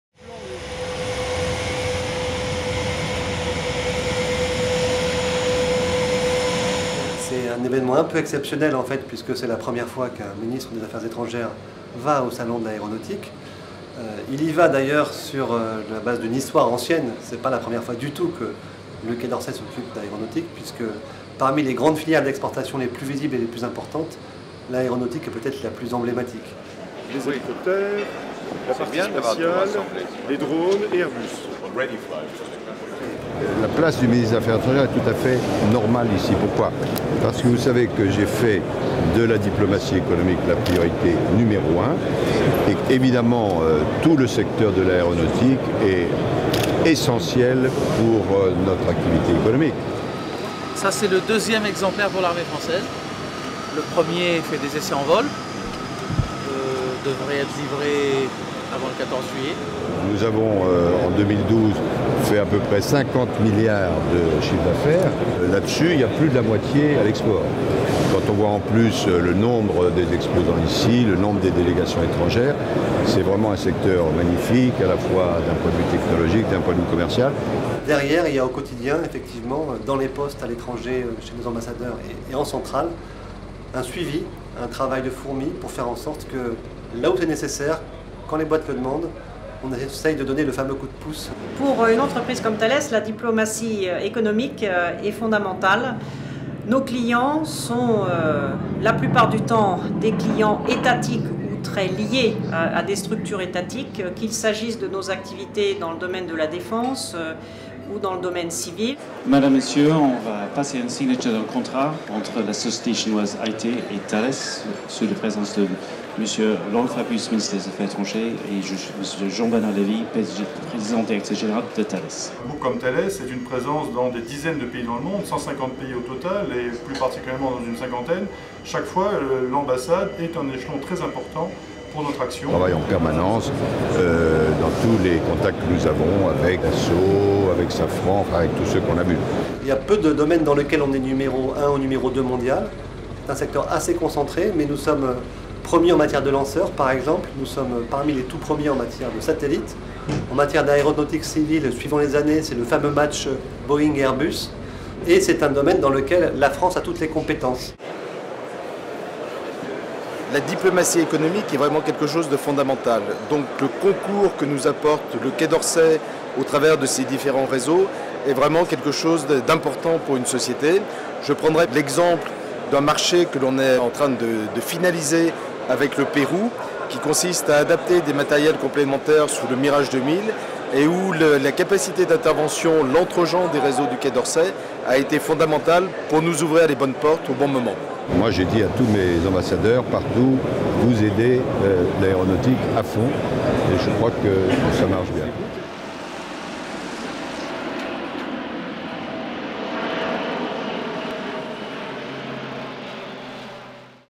Visite de Laurent Fabius au Salon du Bourget (20.06.2013)
Laurent Fabius, ministre des Affaires étrangères, a visité le 20 juin le Salon international de l'aéronautique et de l'espace du Bourget, à l'invitation de Jean-Paul Herteman, président du groupement des industries françaises aéronautiques et spatiales et président-directeur général de l'entreprise Safran.

Instragram : @francediplo